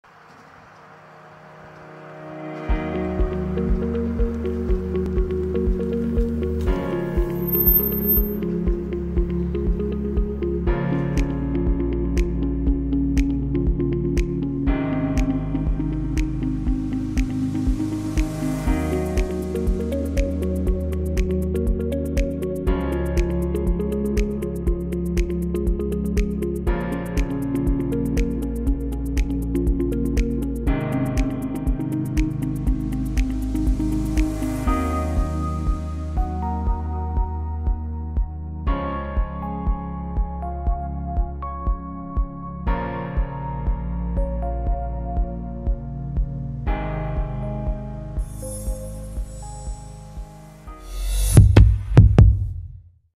A3 Family | Curb View Mirror Programming Tutorial
Learn how to use a curb view mirror when backing into a parking space.